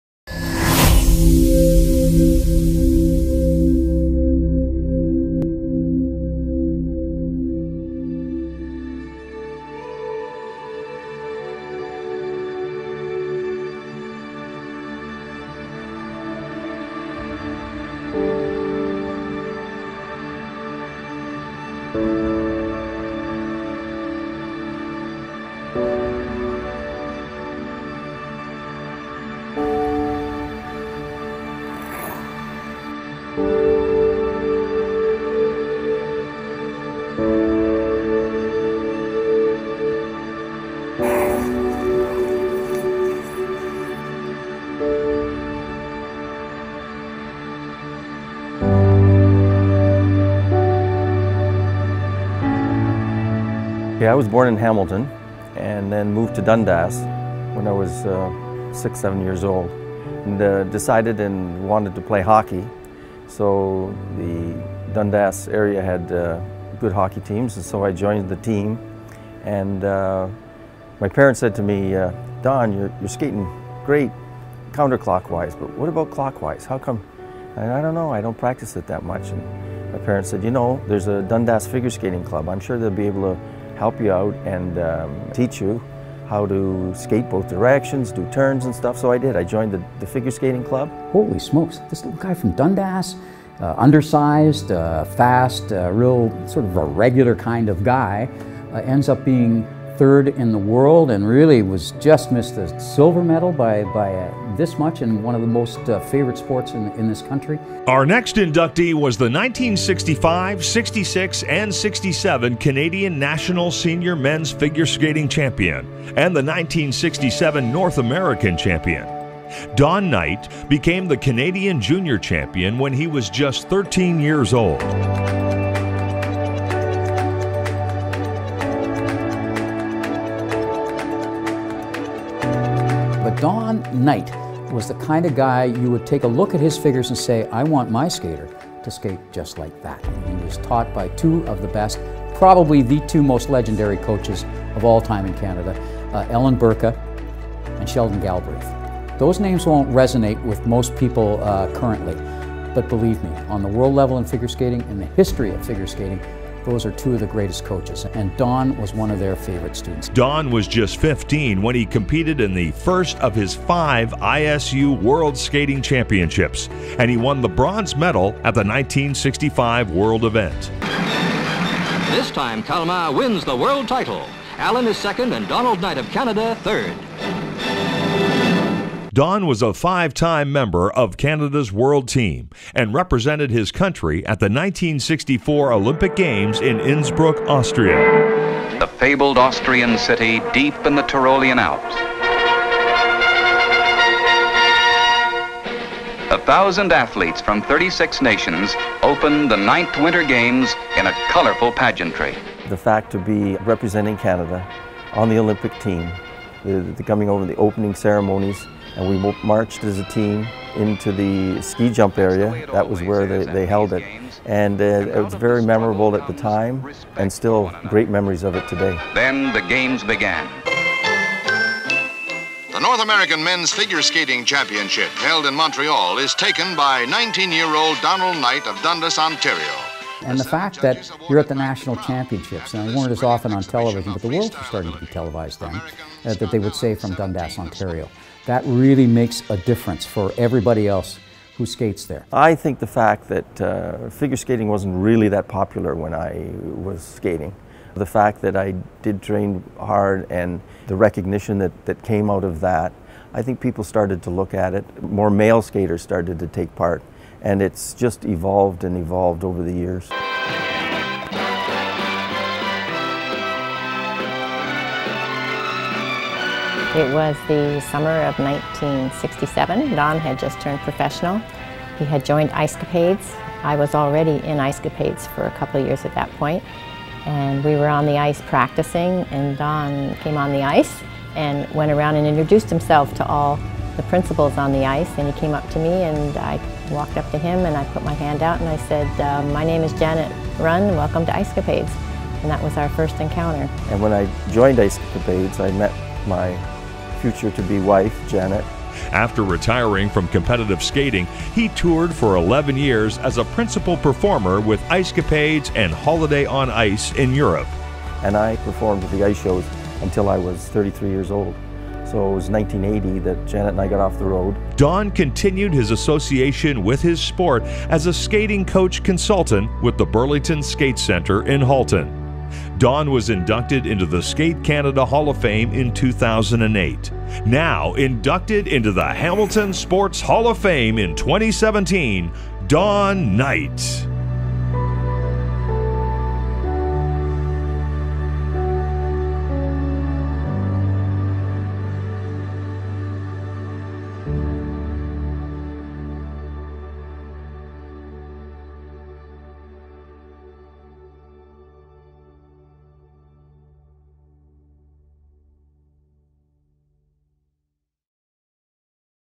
Don Knight
Don was the 1965, ’66 and ’67 Canadian national senior mens’ figure skating champion and 1967 North American champion.
Don, born in Hamilton and raised in Dundas, became the Canadian Junior Champion when he was just 13 years old. He finished on the podium for the next six years at the Senior Canadian Championships.

Don was just 15 when he competed in the first of his five ISU World Skating Championships and he won the bronze medal at the 1965 world event.

Don was a five time member of Canada’s World Team, and represented his country at the 1964 Olympic Games in Innsbruck Austria.

Considered a master of the compulsory figures, Don trained with Hall of Fame coaches Ellen Burka and Sheldon Galbraith and his tremendous work ethic enabled him to become an all-round skater, incorporating powerful jumps, spins and intricate footwork into his programmes.

After retiring from competitive skating, he toured for eleven years as a Principal Performer with Ice Capades and Holiday on Ice in Europe.

After retiring from his professional career, Don continued his association with his sport as a skating coach consultant with the Burlington Skate Centre in Halton.